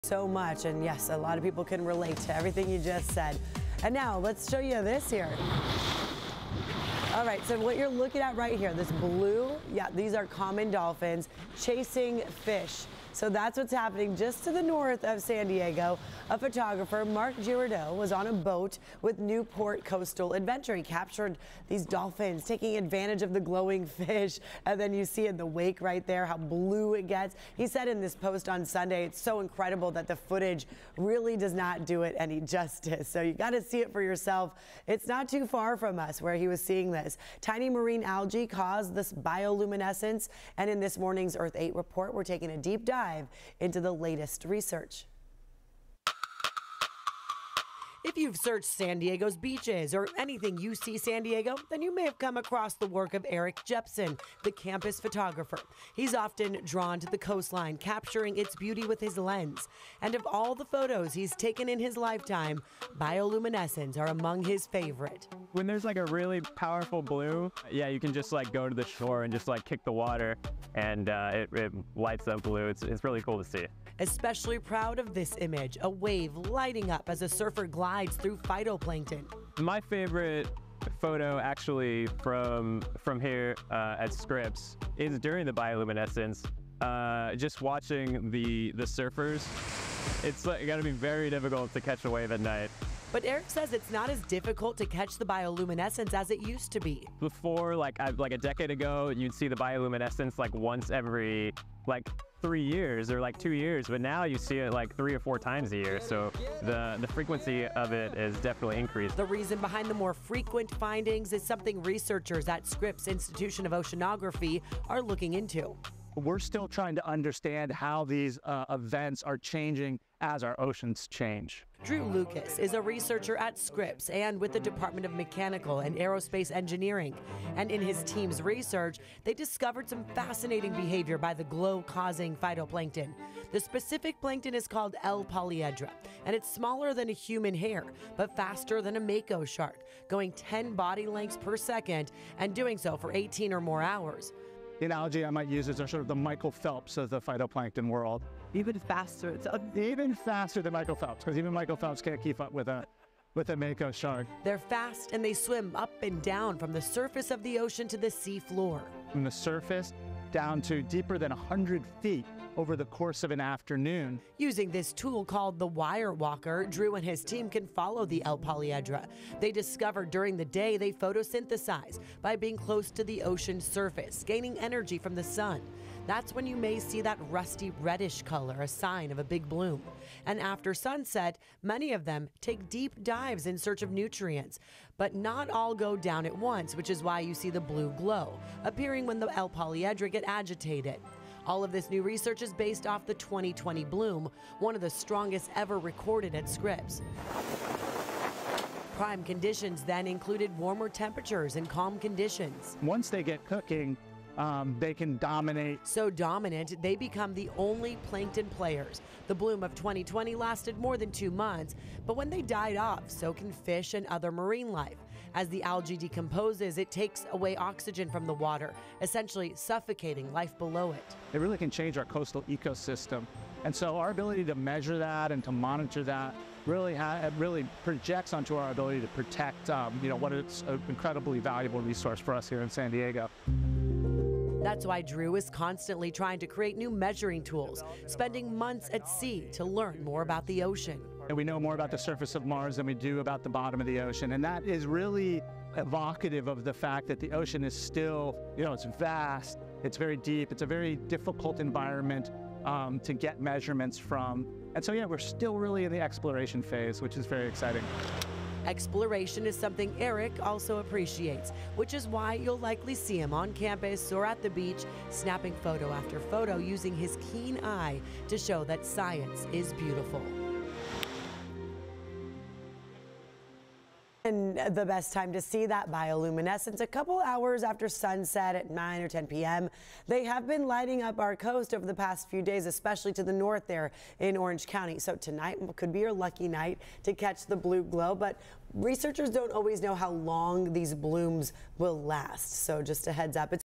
Why does the ocean glow blue? | Bioluminescence in San Diego
The reason behind the more frequent bioluminescence sightings is something researchers at Scripps Institution of Oceanography are looking into.